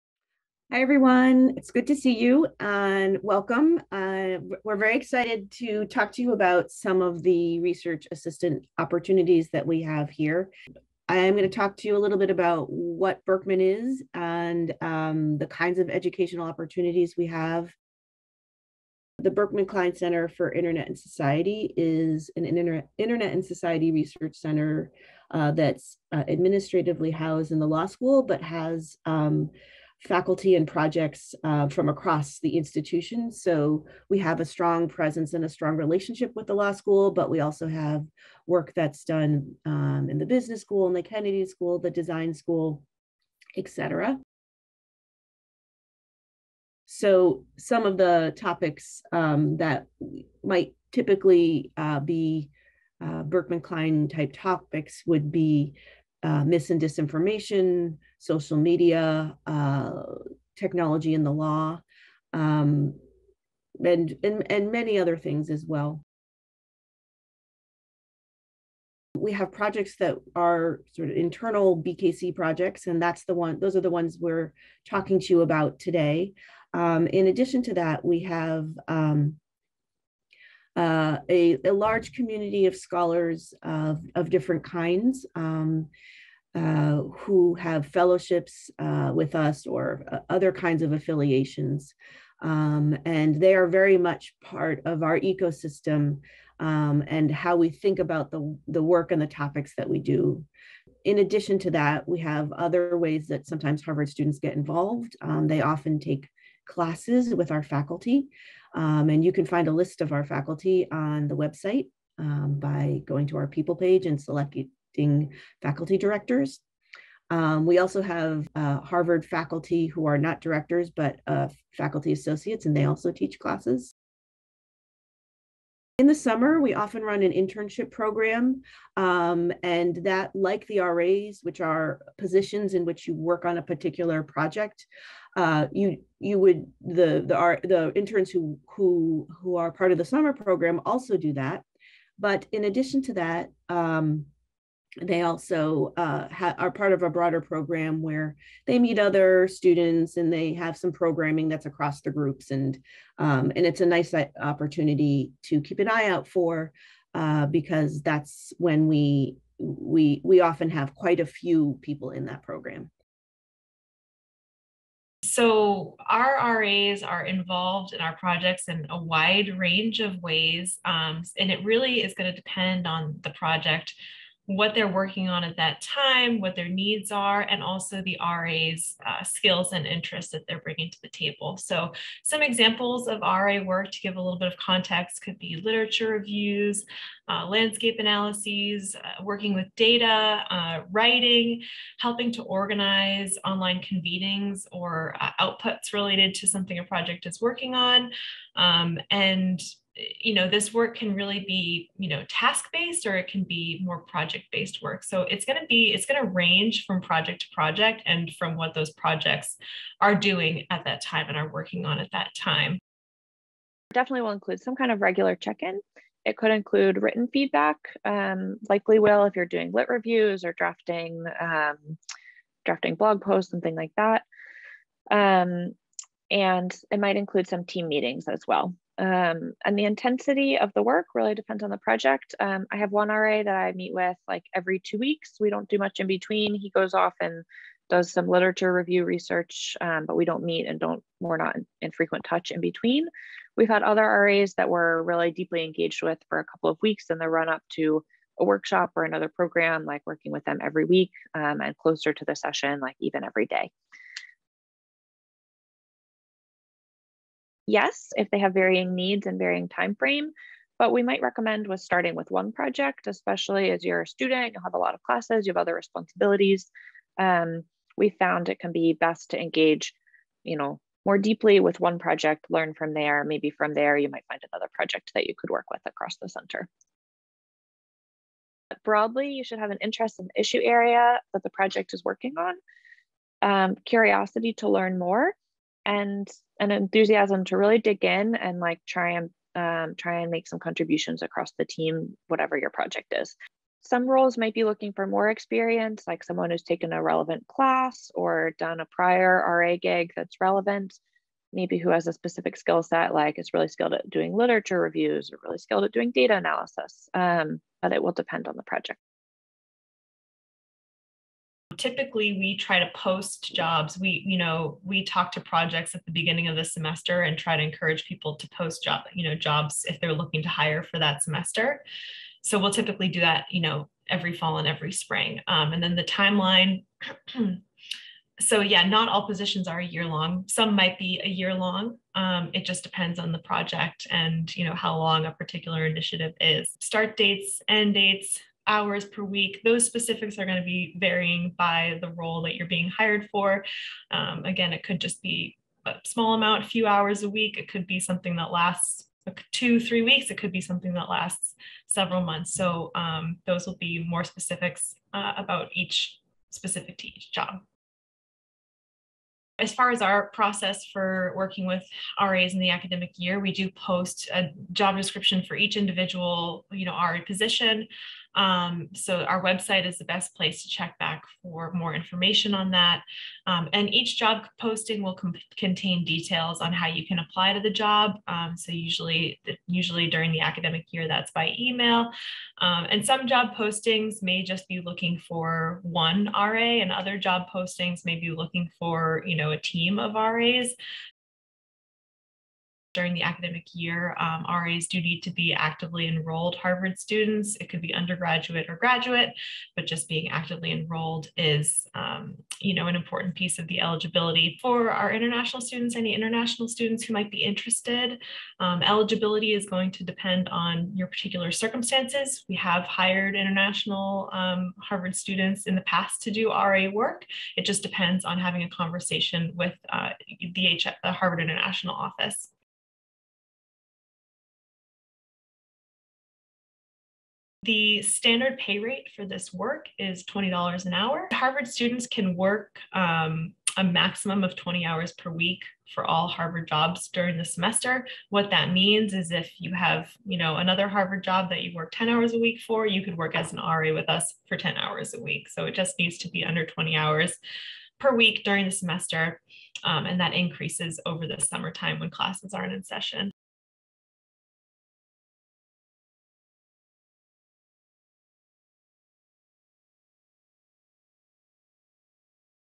1 25 FINAL RA Session
Are you a Harvard student interested in applying to be a Research Assistant for the Berkman Klein Center for Internet & Society?  Watch this video for answers to general questions about the Research Assistantships at Berkman Klein.